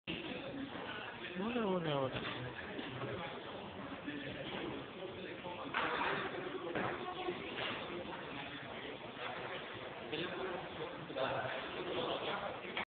En el sushi ruls
Mi amada y yo esperando una infinidad de tiempo a que nos trajeran nuestro fushi